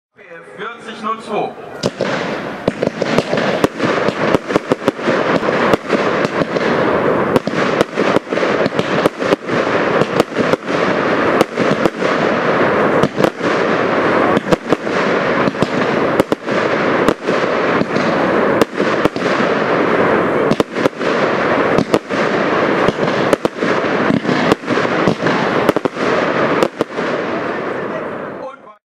Pyrocentury - Red Strobe With Red Tail + Blue Stars (APF 4002)
Gezeigt wurde dieser Artikel auf dem Vorschiessen von AP-Feuerwerk in Übach-Palenberg am 23. Dezember 2017.